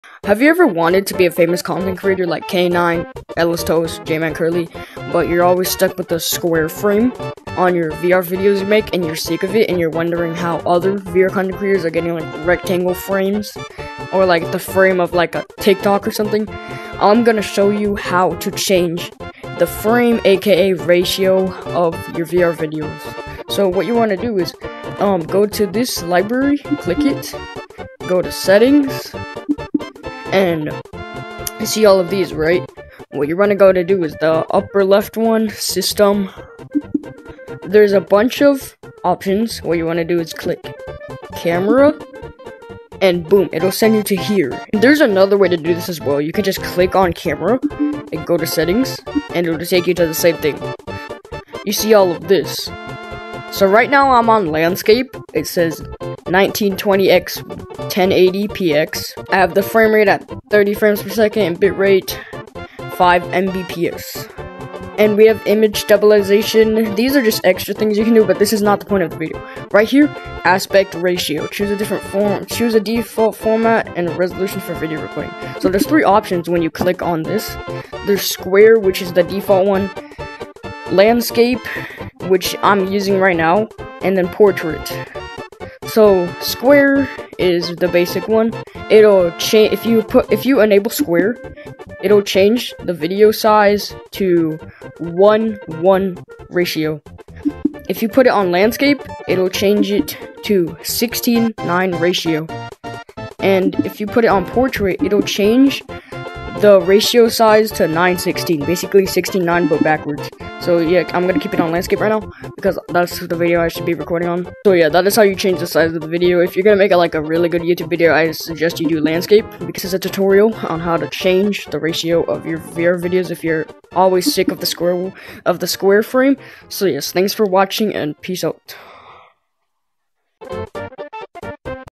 HOW TO CHANGE META QUEST ASPECT RATIO | Tutorial
This is a tutorial video on how to change the frame/size, or ratio of a VR video.

This is for beginners who are sick of the square frame when recording.

There are three ratio options.

-Square (1:1) Used For YouTube Instagram Facebook etc.

-Landscape (16:9) For YouTube Videos
Portrait (9:16) For YouTube Shorts Or TikTok